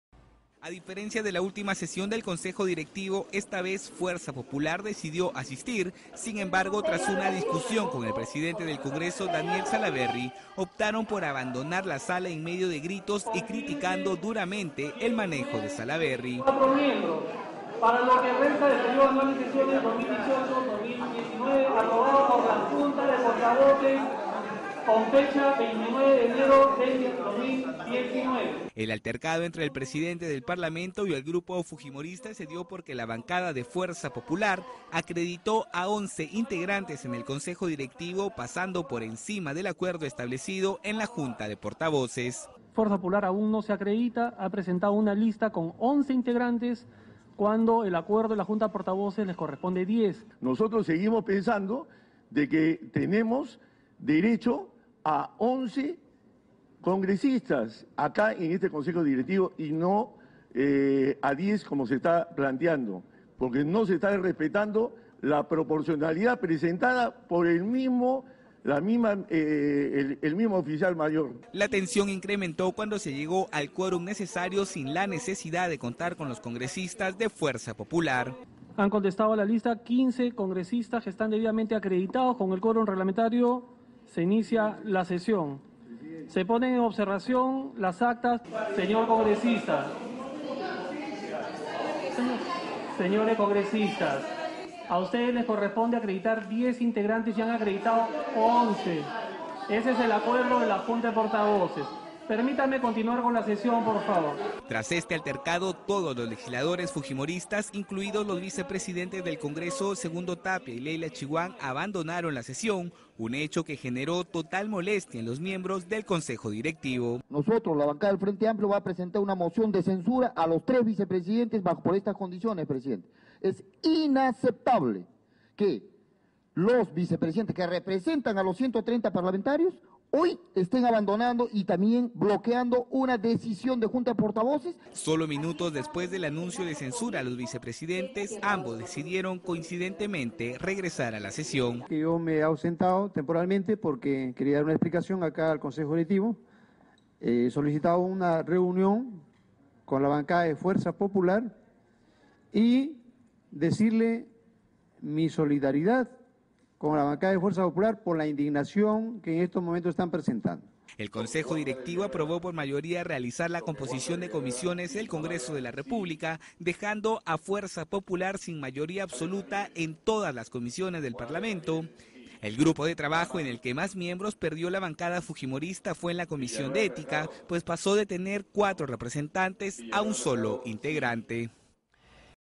Consejo Directivo del Congreso aprobó recomposición de comisiones parlamentarias
Con 13 votos a favor, tres en contra y una abstención, el Consejo Directivo del Congreso aprobó esta mañana la propuesta para recomponer las comisiones ordinarias, así como las extraordinarias del parlamento.

El grupo parlamentario aprobó esta medida luego de una accidentada sesión en la Sala Grau del Congreso, donde hubo intercambio de palabras e integrantes de la bancada de Fuerza Popular terminaron abandonando la reunión.

Segundo Tapia, Leyla Chihuán y el congresista de la Célula Parlamentaria Aprista, Mauricio Mulder, fueron los tres parlamentarios que votaron en contra de la medida tras la creación de nuevas bancadas en el parlamento.

Cabe resaltar, que el vocero del Frente Amplio, Humberto Morales, anunció la presentación de una moción de censura en contra de los los vicepresidentes Segundo Tapia y Leyla Chihuán, quienes dejaron la sesión sin quórum.

Sin embargo, llegó la tercera vicepresidenta Yeni Vilcatoma, con quien se logró tener el número legal para poder iniciar la sesión. Tras varios minutos, tanto Segundo Tapia como Leyla Chihuán se reintegraron a la sala.